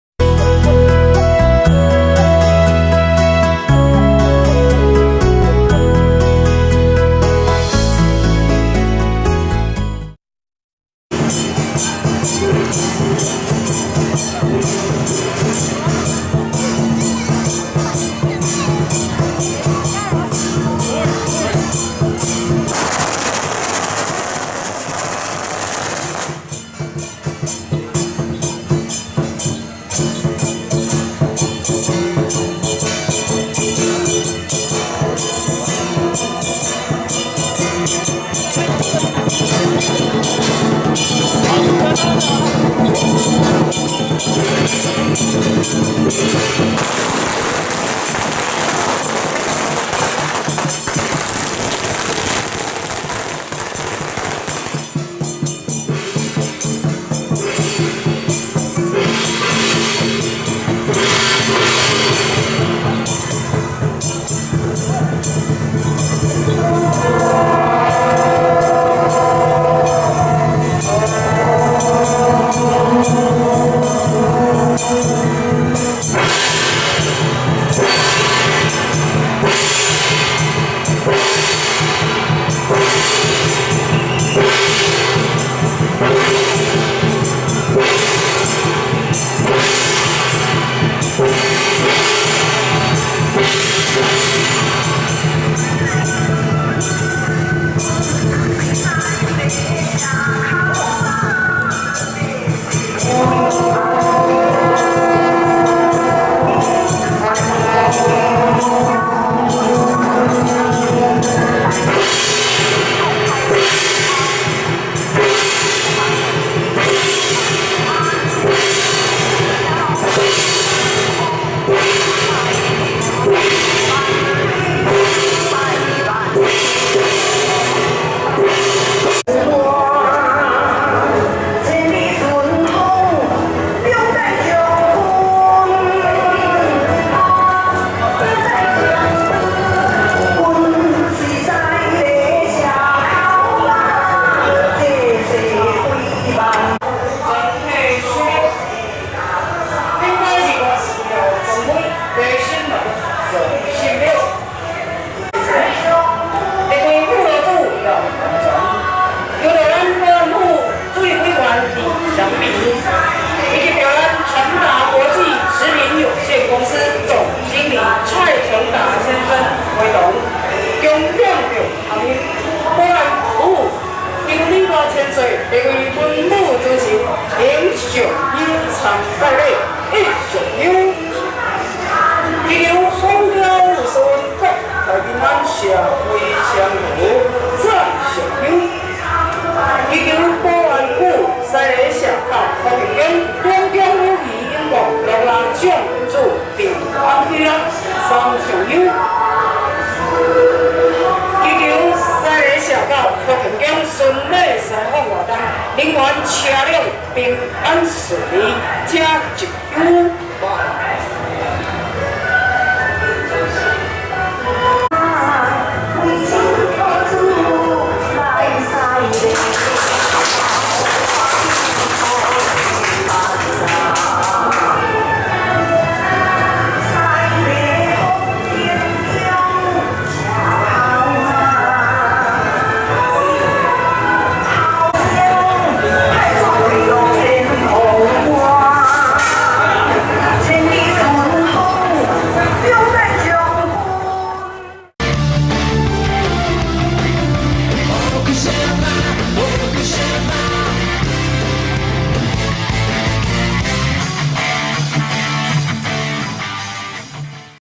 元長鄉三房寮 保 安 府 ~張李莫府千歲~停駕參禮@西螺福天宮 山海感恩 巡禮西訪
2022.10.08 壬寅年 九月十三日
西螺福天宮 社口媽
雲境山海 感恩賜福 巡禮西訪 會香